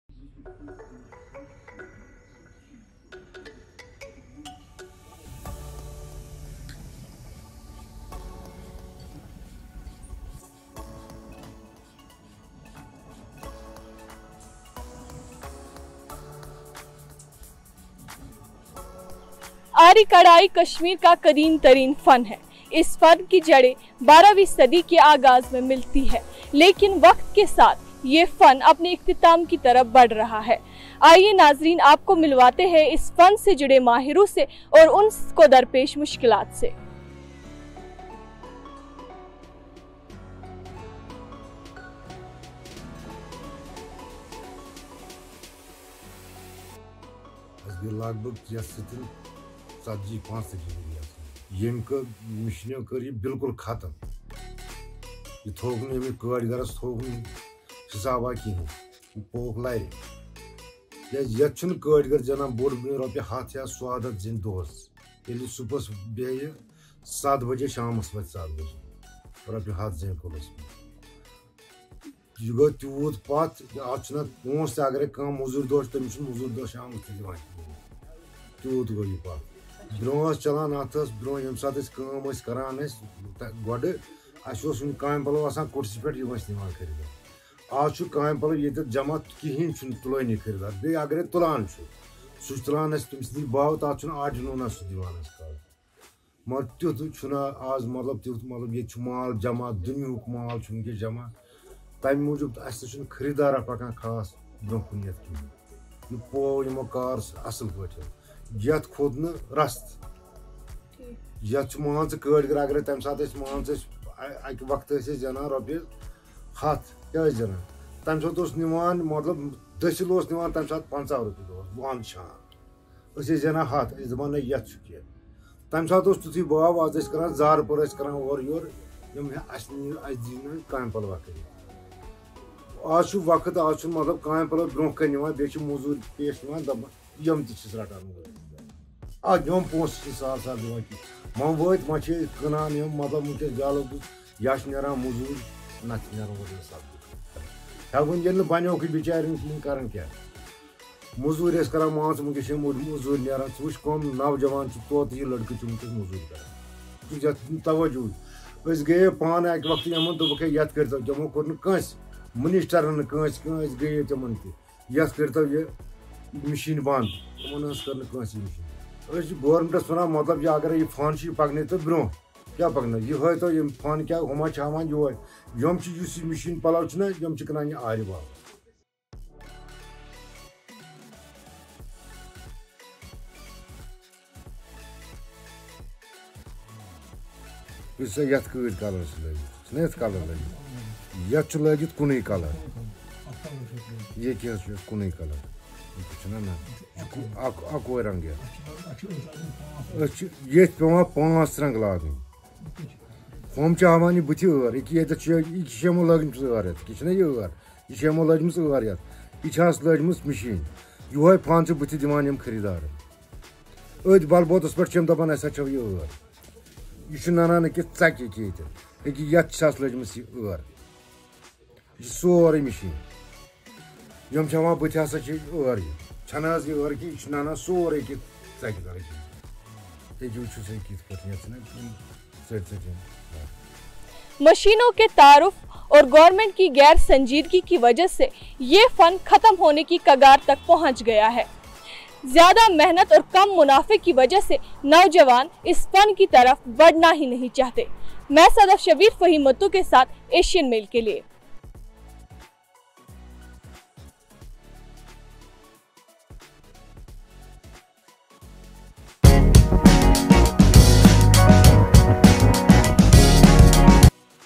Aari Work: A Tale of Enchantment | AMC
Aari is named after the awl or needle used by craftsmen known as the 'Aar'. An artisan carefully traces the pattern on transparent paper. Fabric is placed on the paper after it has been perforated along its outline. Ink is imprinted on the fabric below by rubbing a special chalk over the paper. A rectangular, wooden frame is mounted on the fabric, and embroidery begins.

Reports: Sadaf Shabir
Edit: Aqib Nazir